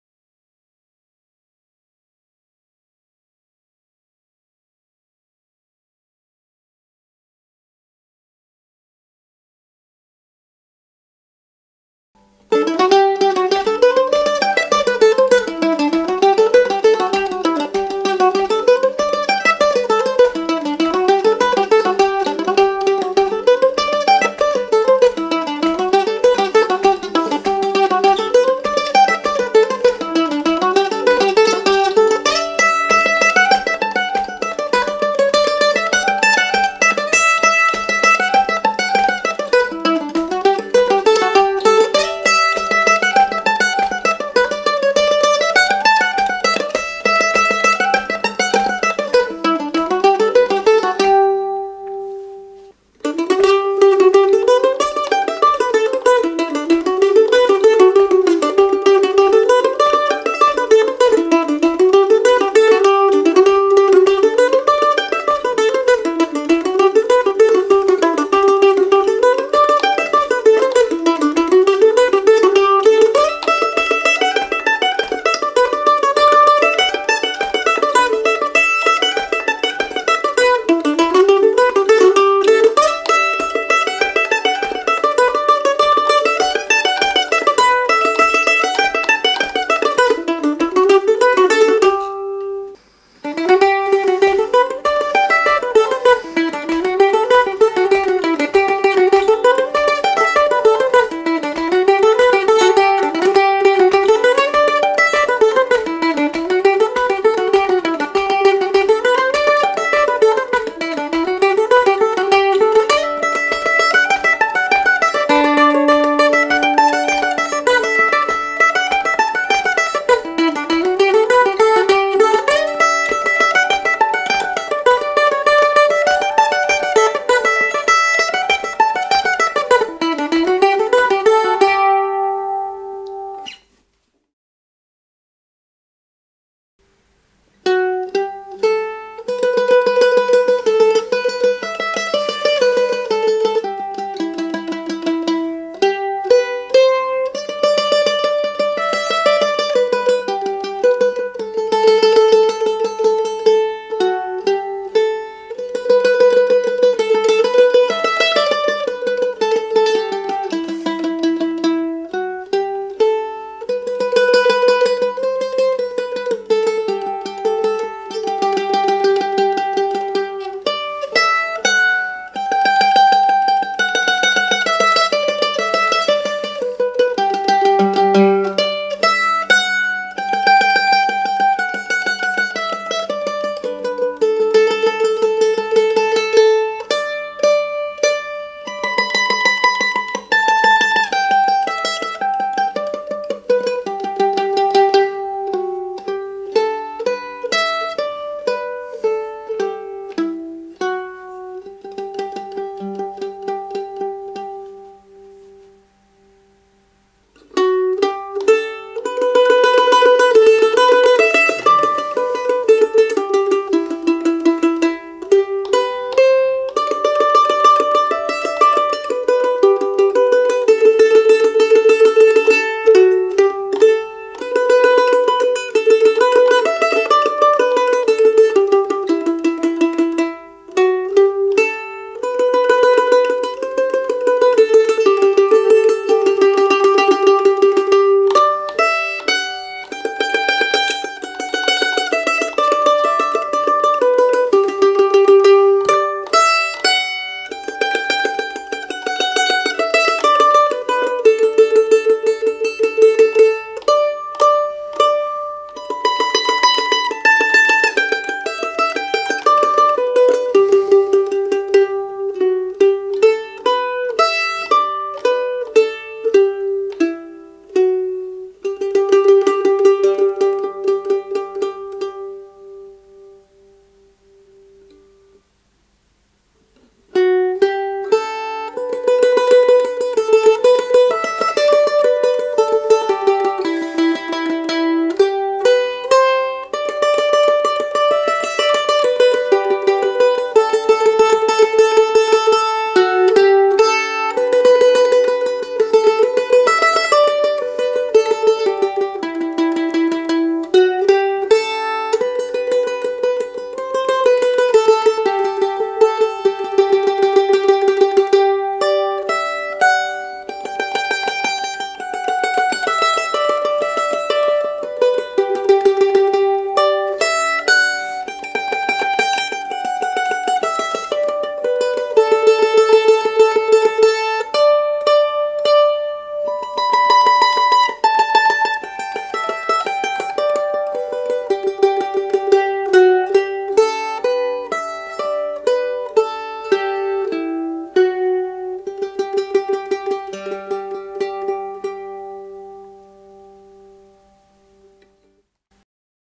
Mandolin comparison: Gibson A (1920), Gibson F5-L (1980) vs. Oakbridge (2015)
I inherited two excellent Gibson mandolins (A and F models), and I allready had a very good Oakbridge mandolin made by finnish luthier Jouni Järvinen. I know its really silly to own three such a good instruments, but they all sound great but different that I have difficulty letting go of any of them.

Here's my personal feeling about the instruments:

Gibson F5-L is maybe easiest to play, its loud and has fast attack. Its easy to understand why bluegrass players like it. Its also clearly my choise for (rhytmic) accompaniment.

Oakbridge and Gibson A model sound somewhat similar (I know that Jouni has studied old A-models carefully). Gibson has this wonderfull "old" sound, its darker than the Oakbridge. Oakbridge mandolin has brighter sound and also very good sustain. In my opinion there is also certain "depth" in the sound of Oakbridge that I cannot hear from other two. Oakbridge mandolin has clearly wider neck than the other two, which is of course a matter of opinion if you like it or not. Its also the only one of these mandolins with inbuild microphone.